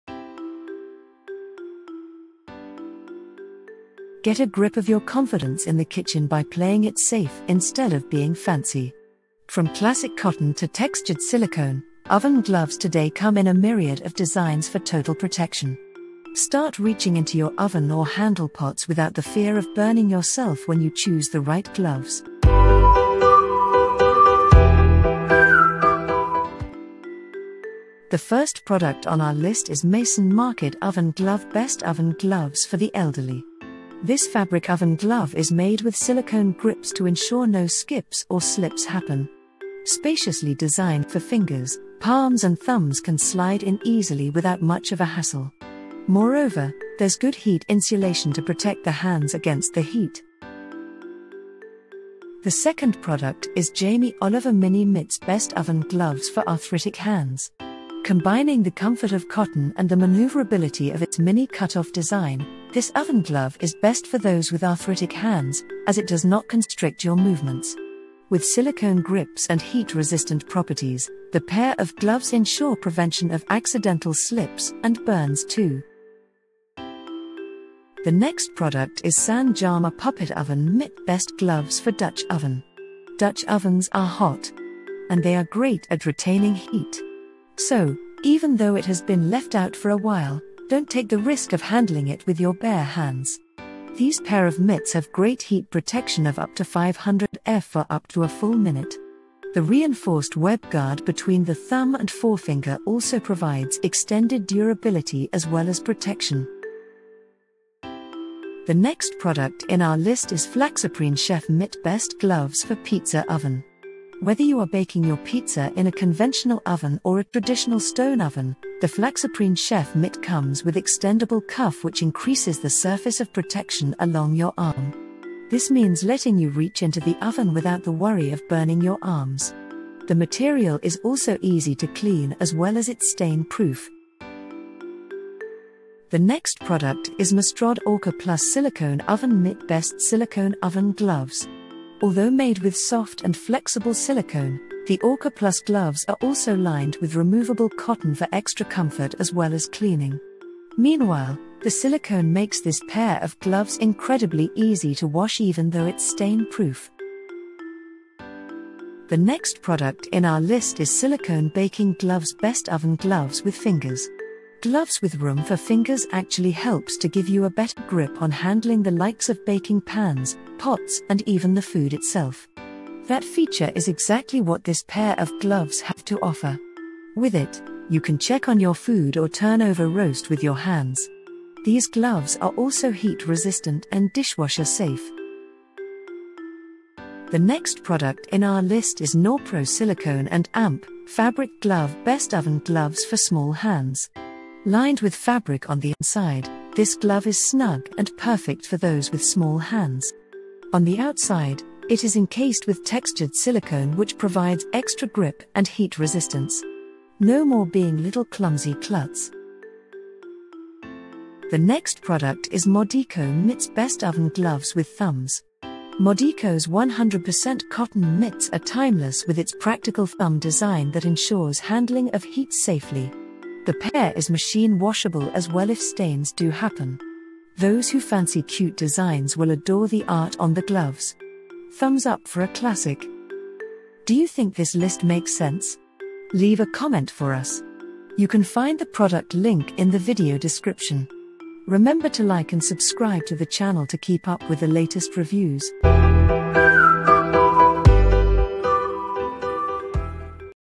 🌵 8 Best Oven Gloves in Singapore
🌵 8 Best Oven Gloves in Singapore | Lemongrass. Here are our editor picks:

00:00 Introduction

00:26 #1 - Mason Market Oven Glove Best oven gloves for the elderly (SGD19.90)

00:50 #2 - Jamie Oliver Mini Mitts Best oven gloves for arthritic hands (SGD21.40)

01:16 #3 - San Jarma Puppet Oven Mitt Best gloves for dutch oven (SGD28.80)

01:46 #4 - Flaxaprene Chef Mit Best gloves for pizza oven (SGD35.00)

02:14 #5 - Mastrad OrkaPlus Silicone Oven Mitt Best silicone oven gloves (SGD25.00)

02:37 #6 - Silicone Baking Gloves Best oven gloves with fingers (SGD10.15)

03:05 #7 - Norpro Silicone & Fabric Glove Best oven gloves for small hands (null)

03:29 #8 - Modeco Mitts Best oven gloves with thumbs (SGD15.90)

03:53 Ending

Get a grip of your confidence in the kitchen by playing it safe instead of being fancy. From classic cotton to textured silicone, oven gloves today come in a myriad of designs for total protection. Start reaching into your oven or handle pots without the fear of burning yourself when you choose the right gloves.